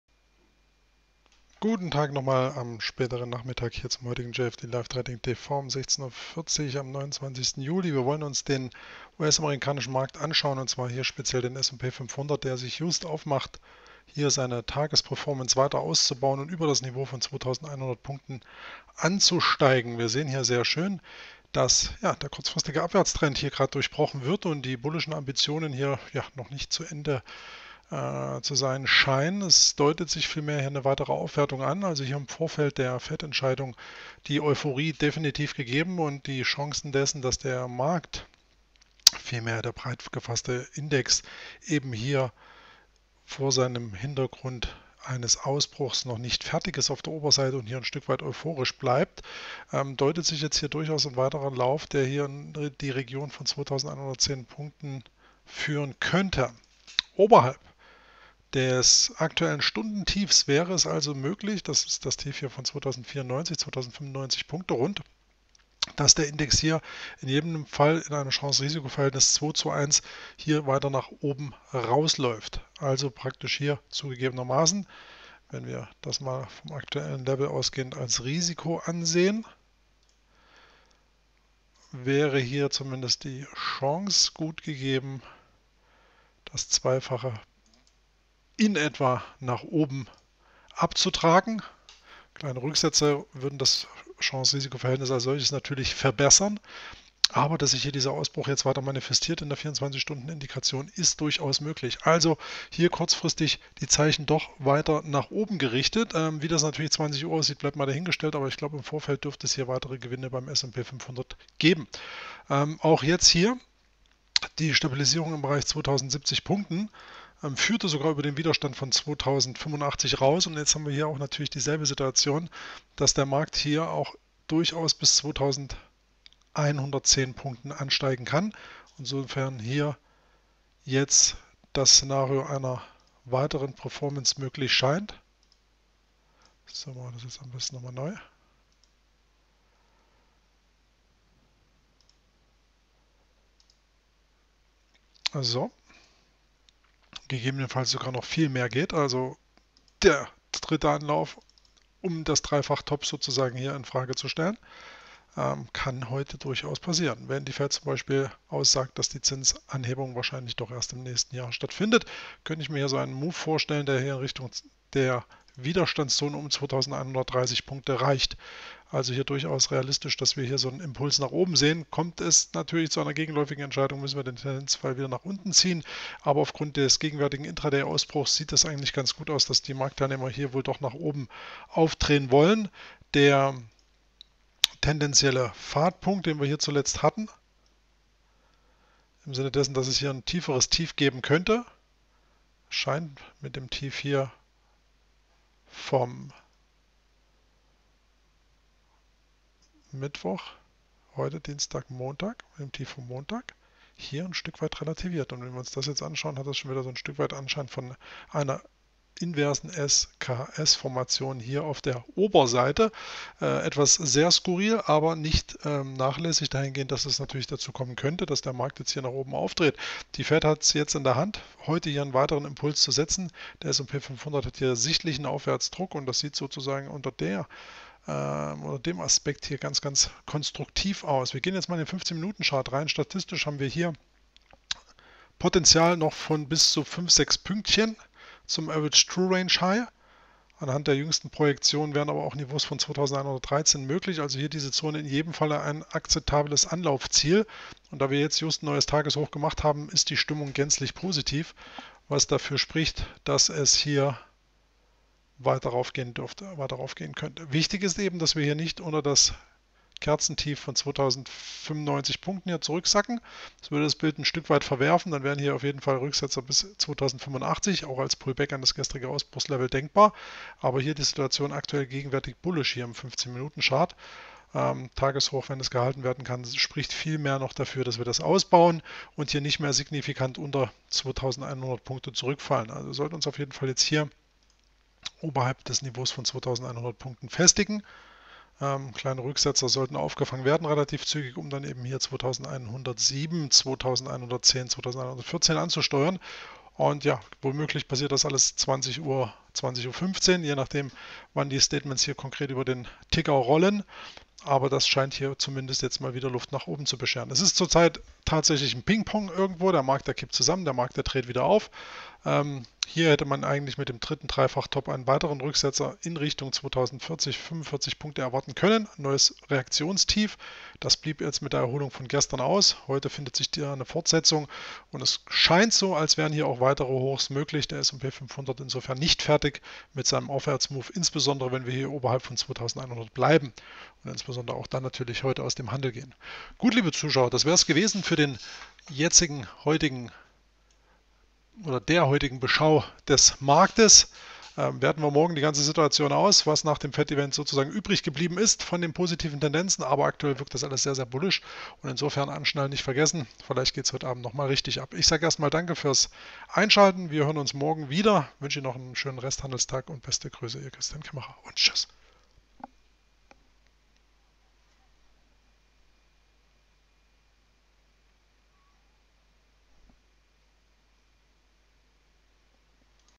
S&P 500 und warum noch mehr gehen könnte
Just überschreitet der S&P 500 Index das Level von 2.100 Punkten und scheint dadurch hungrig auf mehr in Sachen Performance gen Norden. Was möglich sein könnte, erfahren Sie im Clip.